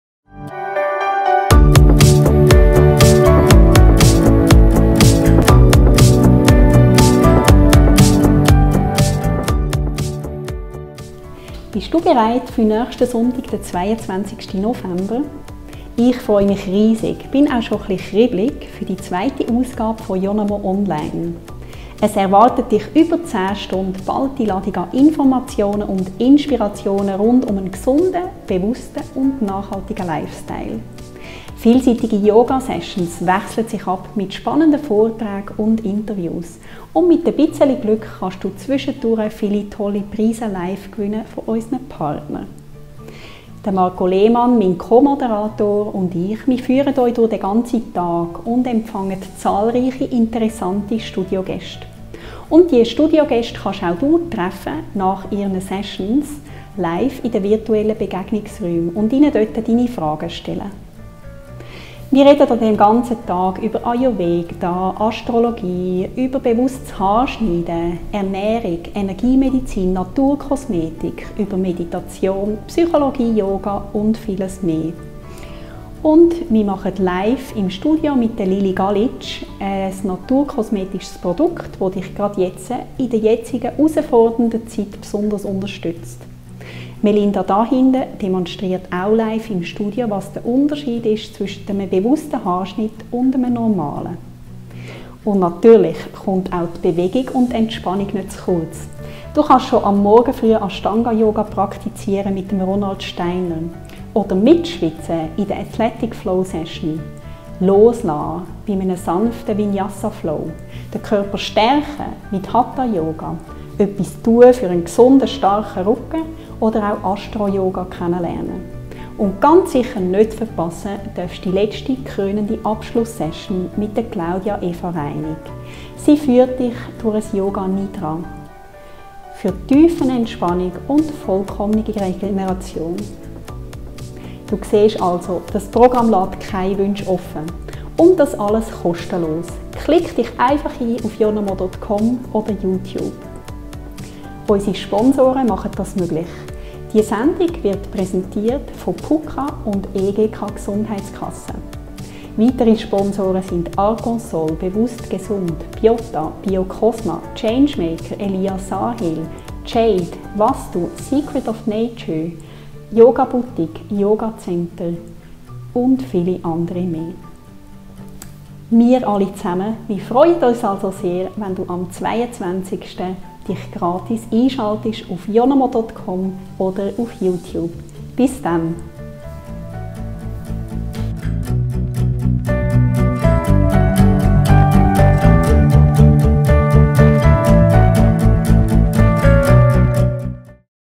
Yonamo Online | 22. November mit Martine Barro
Bist du bereit für Sonntag, 22. November. Ich freue mich auf die 2. Ausgabe von Yonamo Online. Es erwartet dich über 10 Stunden geballte Ladung an Informationen und Inspirationen rund um einen gesunden, bewussten und nachhaltigen Lifestyle. Vielseitige Yoga-Sessions wechseln sich ab mit spannenden Vorträgen und Interviews. Und mit etwas Glück gewinnst du zwischendurch immer wieder mal tolle Preise von unseren Partnern.
Klicke dich kostenlos ein auf YouTube oder und sei dabei!